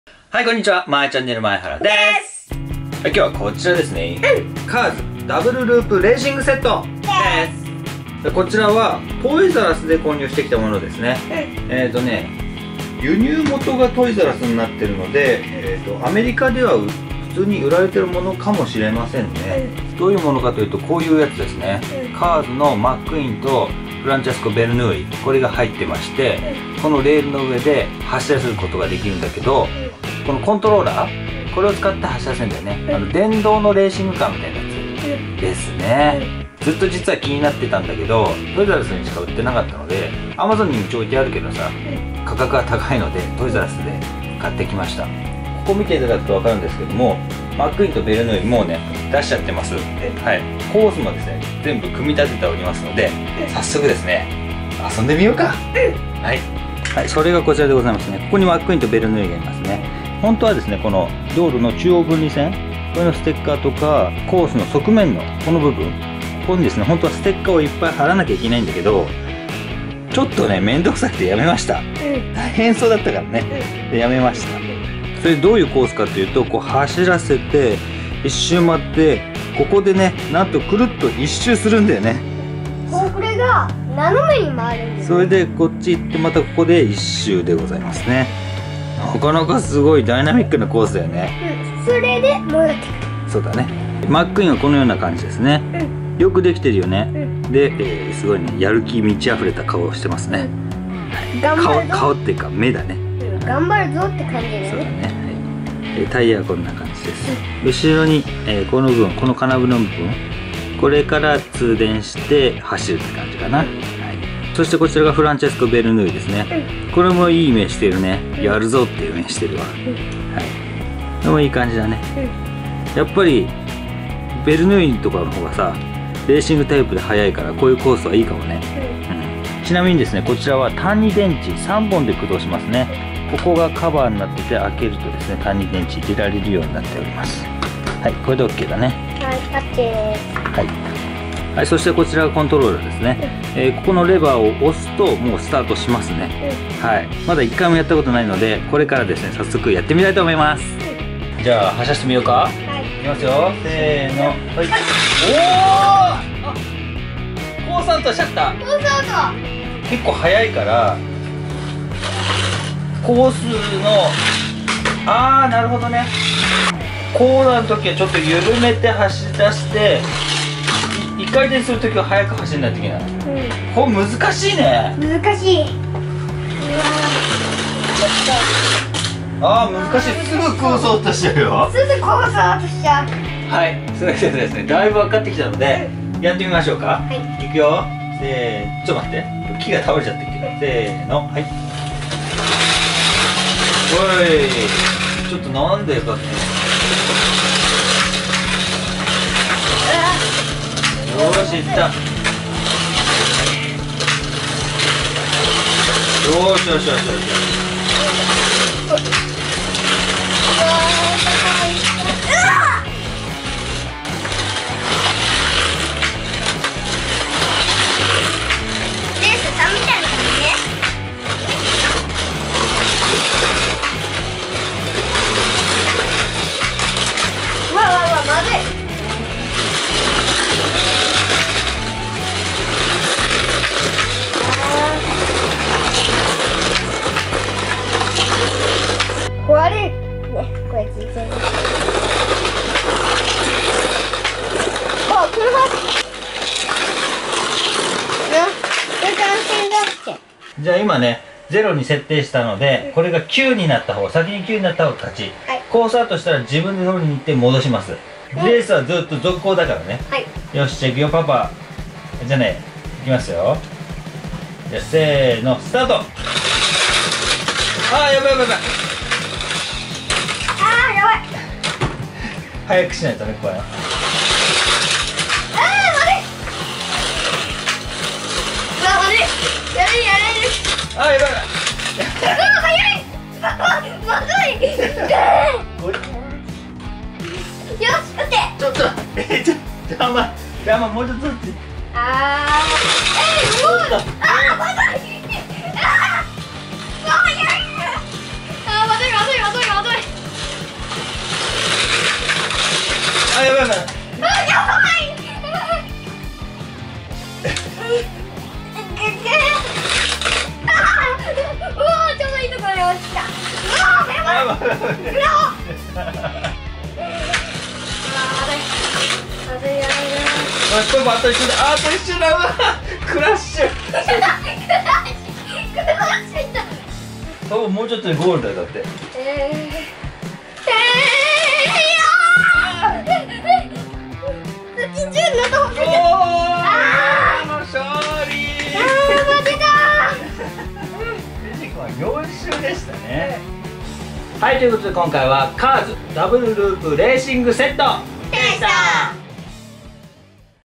対象年齢3歳以上？！ カーズ ダブルループレーシングセット Cars Double Loop Racing Set
カーズ ダブルループレーシングセットを購入しました！
随分前から気になっていた商品だったのですが、トイザらスにしか置いておらず時には在庫がなかったのでようやく購入できました！
ちなみにアマゾンでは非常に高値です^^;

このセットにはライトニング・マックィーンとフランチェスコ・ヴェルヌーイが付属しております(*^^*)

ちなみに対象年齢は3歳以上ですが、非常に難易度が高く安定して走行させるのに練習が必要です！

とは言えとても楽しい商品です！^^

▽カーズ　ダブルループレーシング（トイザらス）

▽使用音楽（YouTube オーディオ ライブラリ）

▽撮影機材　SONY α NEX-5R（Amazon）